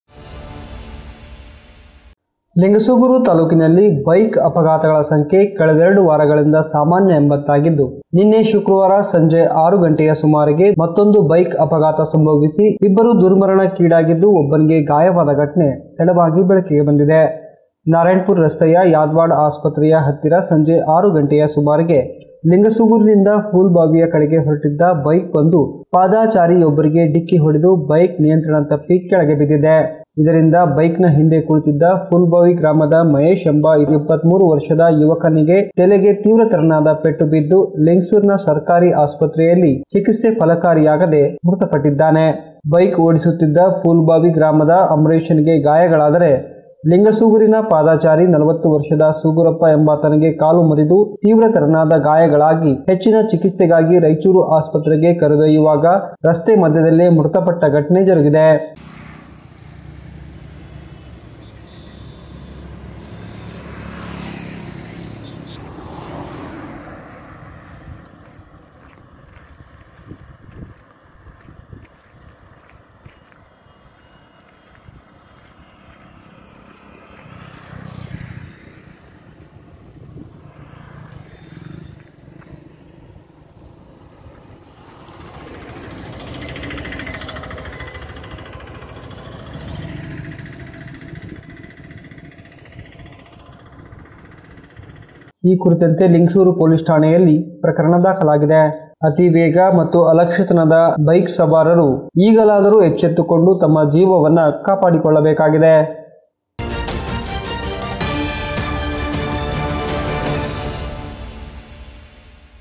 ನಿಲ್ಲದ ಬೈಕ್ ಅಪಘಾತ ಇಬ್ಬರ ಸಾವು ಒಬ್ಬನಿಗೆ ಗಾಯ Lingasugur news SLVC NEWS 16 10 2021
SLVCChannel